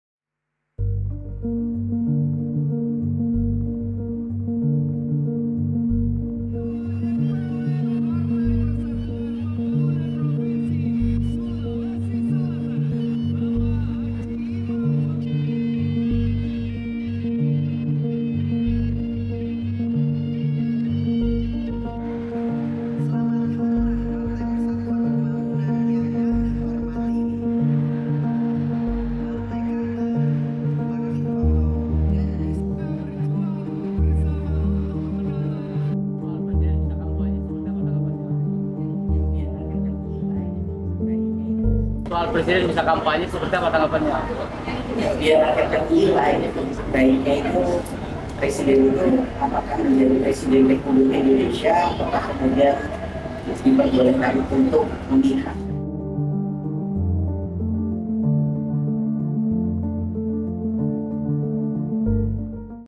Presiden boleh kampanye, Puan: biar rakyat yang nilai
ANTARA - Ketua DPP PDIP sekaligus Ketua DPR RI, Puan Maharani pada saat menghadiri Harlah ke-51 Partai Persatuan Pembangunan (PPP) di Makassar, Sulawesi Selatan, Sabtu (27/1) memberikan tanggapan terkait pernyataan Presiden Jokowi yang membolehkan Presiden berpihak dan ikut berkampanye. Puan mengatakan yang berhak menilai adalah rakyat. (Shintia Aryanti Krisna/Rizky Bagus Dhermawan/Amita Putri Caesaria/Angel Tabitha)